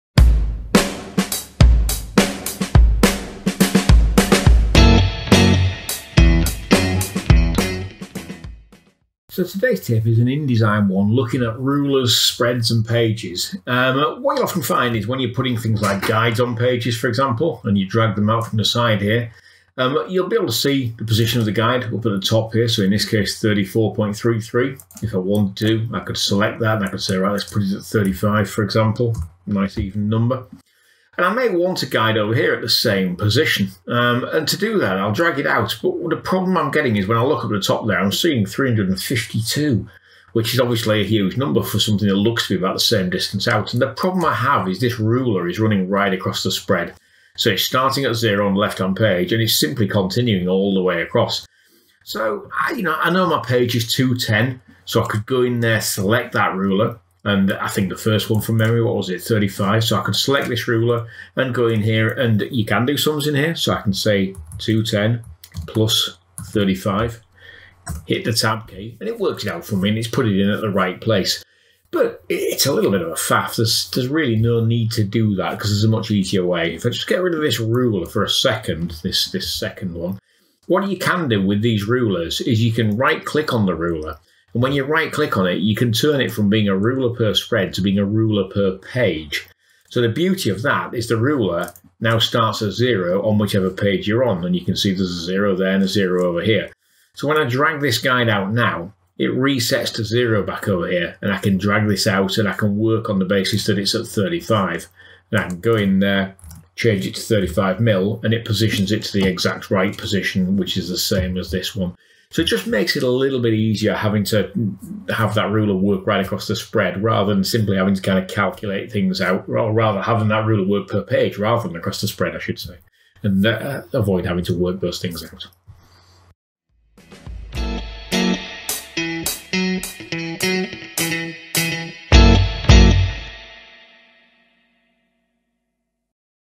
Learn how to work with rulers in InDesign
Find out how to add, position, and move rulers in InDesign to make your work more accurate and easier to position things.